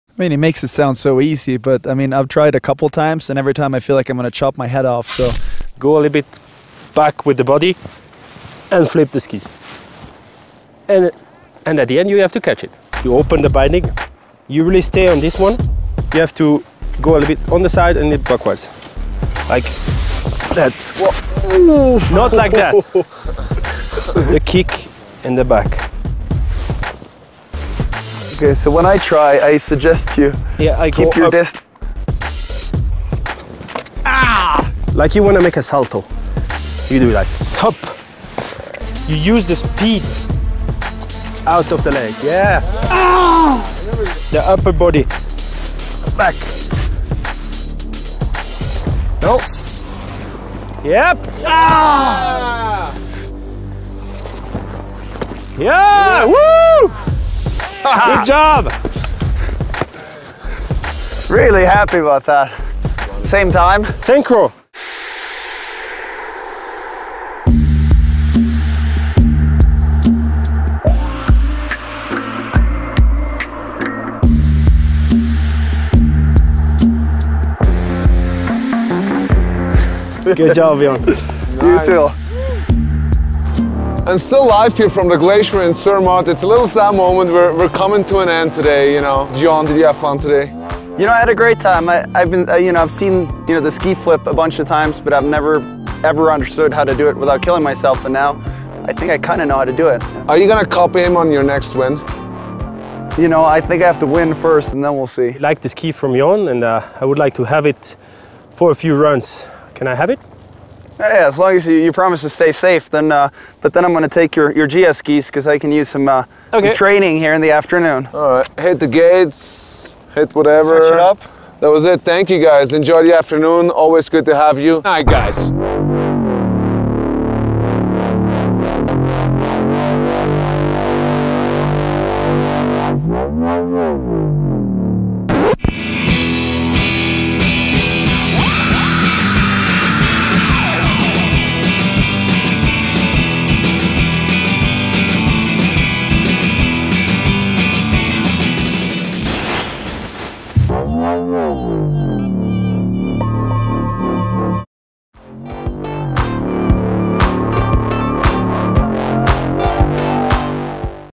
HEAD Skiers United - Episode Two
SKIERS UNITED! Didier Cuche tries to teach Jon Olsson a ski flip. Can he do it without getting hurt (or hurting the camera man)? More challenges for our "crossover" ski team coming up soon